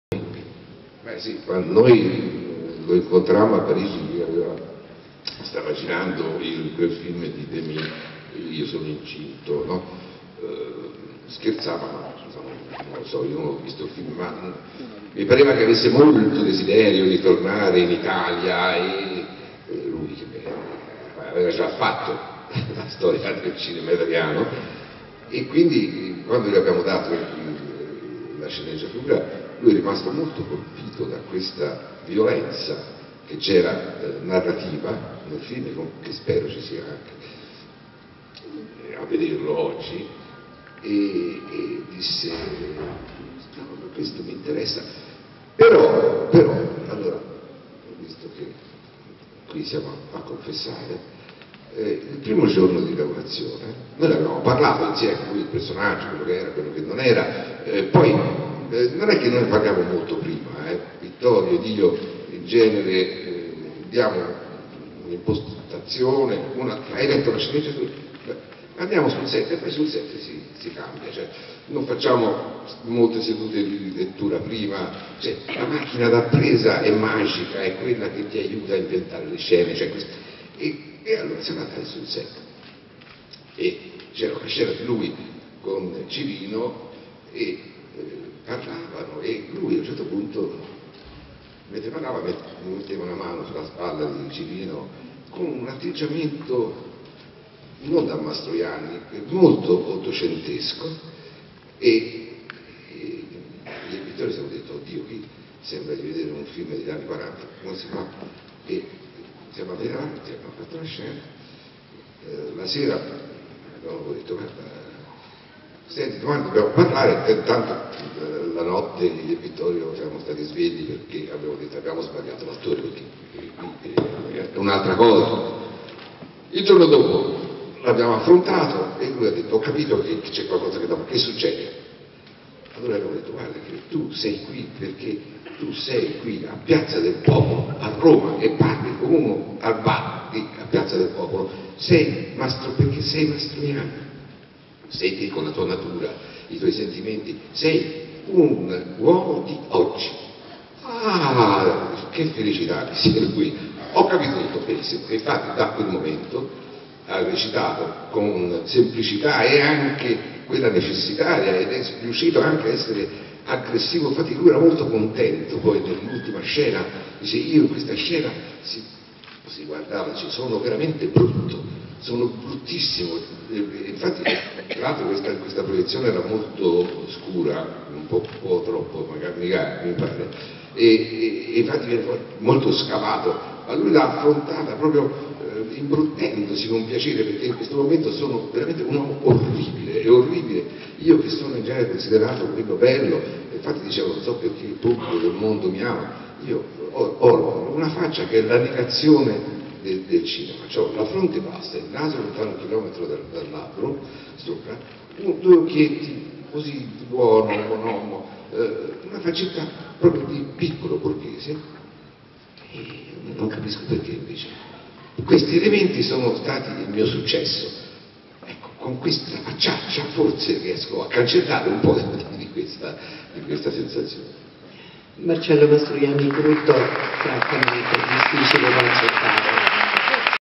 Paolo Taviani: il personaggio di Marcello Mastroianni in Allonsanfàn
Nella carriera di Marcello Mastroianni, il personaggio di Fulvio Imbriani è forse quello meno simpatico, quello del traditore, e l'attore non ha esitato molto ad accettare. Lui raccontava sempre, infatti, che dopo il latin lover aveva voglia di fare tutt'altro per cancellare quell'immagine e costruire un’altra immagine di attore. In questo video Paolo Taviani, al BIF&ST 2016 per una masterclass a lui dedicata, racconta come, dopo le incertezze delle prime riprese, Marcello Mastroianni non abbia avuto difficoltà ad interpretare il suo personaggio con naturalezza e senza troppe infrastrutture.